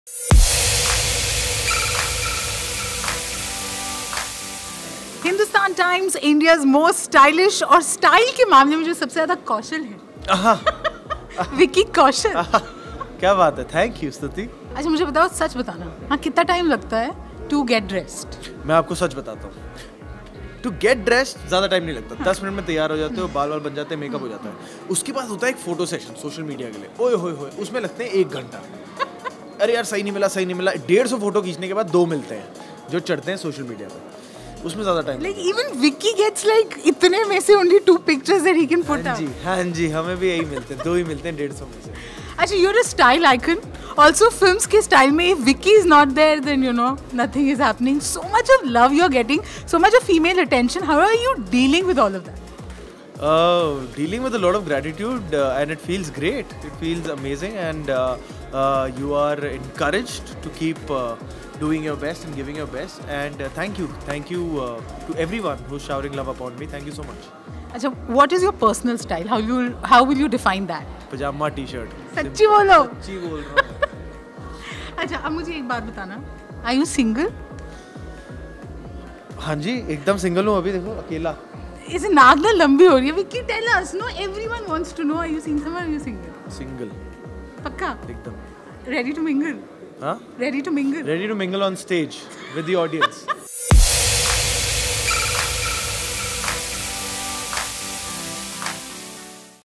Vicky Kaushal clears the air on his relationship status
We asked Uri: The Surgical Strike actor Vicky Kaushal his current relationship status. Is he single or taken? At the Hindustan Times India's Most Stylish 2019 in Mumbai RJ Stutee Ghosh spoke to Vicky Kaushal, who hosted the event along actor Radhika Apte. Vicky won the Jeep Badge of Honour in recognition of his fashion and style. We asked Vicky what he thinks of that and how long does he take to dress up. Catch the full conversation in this quirky video.